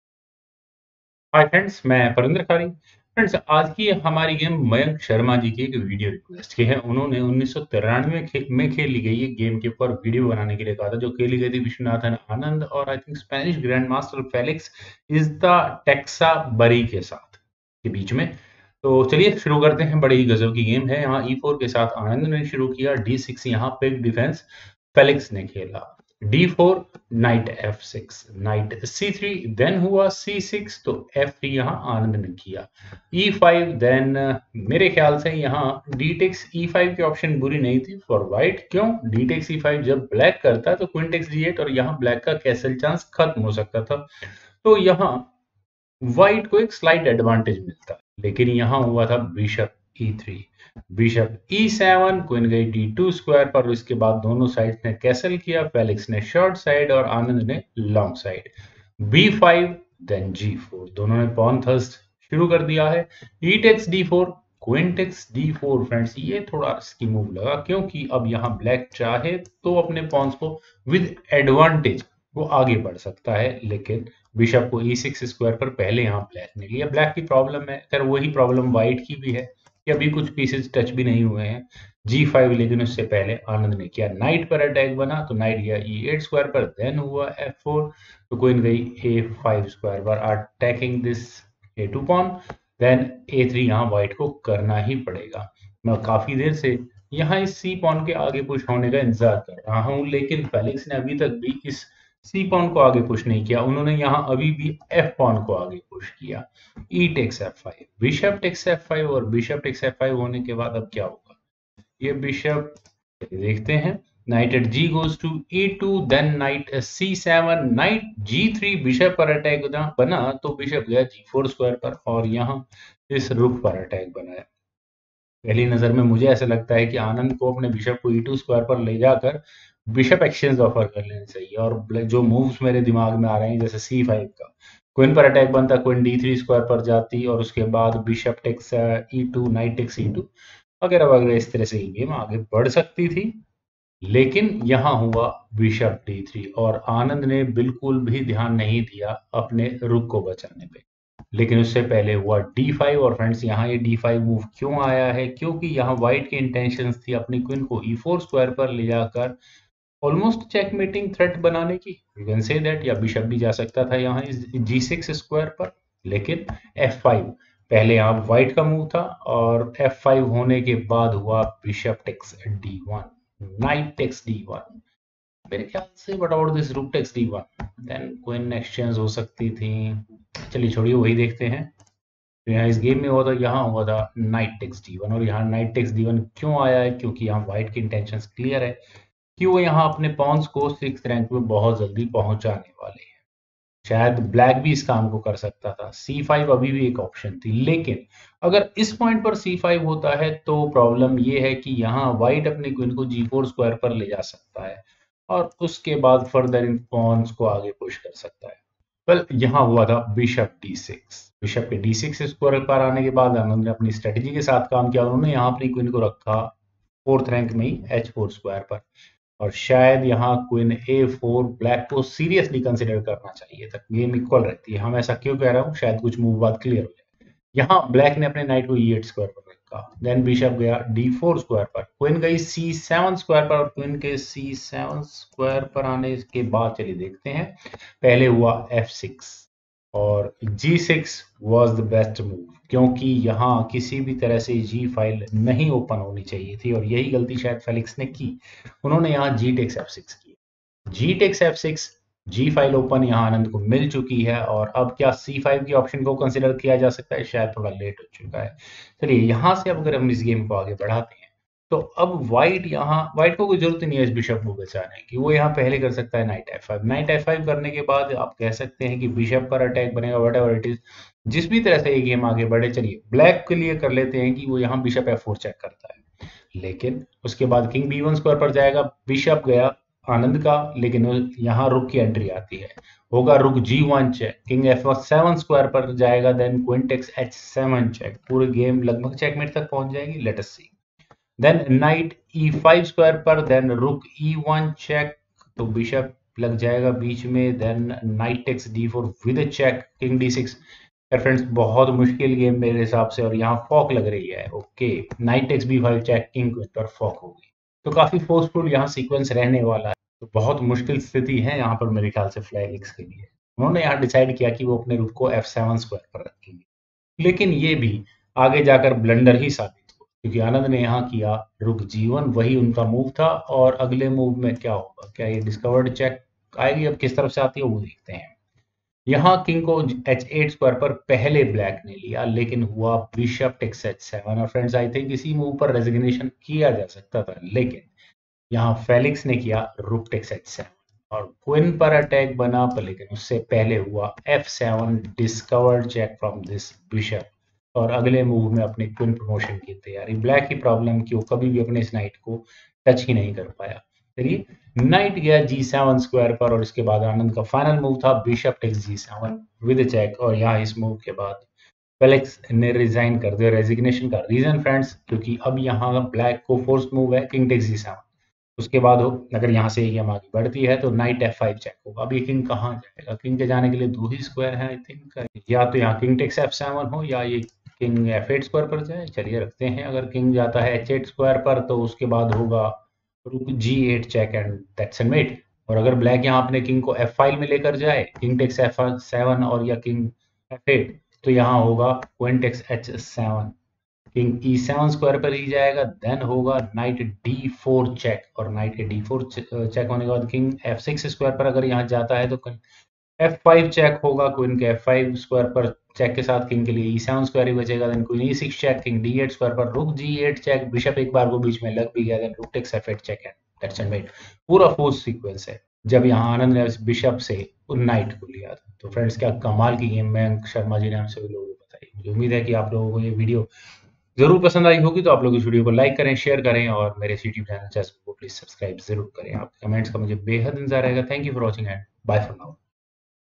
Anand's Fierce Attacking game
Hello and welcome! My name is ParvinderKhari and in this video, we will talk about the game played between Viswanathan Anand vs Felix Izeta Txabarri in 1993
[Event "Madrid"]
[Site "Madrid ESP"]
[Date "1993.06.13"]
[EventDate "?"]
[Round "7"]
[Result "1-0"]
[White "Viswanathan Anand"]
[Black "Felix Izeta Txabarri"]
[ECO "B07"]
[WhiteElo "?"]
[BlackElo "?"]
[PlyCount "51"]
1.e4 d6 2.d4 Nf6 3.Nc3 c6 4.f3 e5 5.Be3 Be7 6.Qd2 O-O 7.O-O-O b5 8.g4 exd4 9.Qxd4 Be6 10.g5 Ne8 11.f4 Qa5 12.a3 f5 13.exf5 Bxf5 14.Nge2 Nc7 15.Ng3 Bg4 16.Bd3 d5 17.f5 Bxd1 18.Nxd1 Bd6 19.Qh4 Ne8 20.Bd4 Qc7 21.f6 gxf6 22.gxf6 Rf7 23.Rg1 Kh8 24.Bxh7 Rxh7 25.f7 Ng7 26.Bxg7+ 1-0
Download the android game
2048 Real Brain Teaser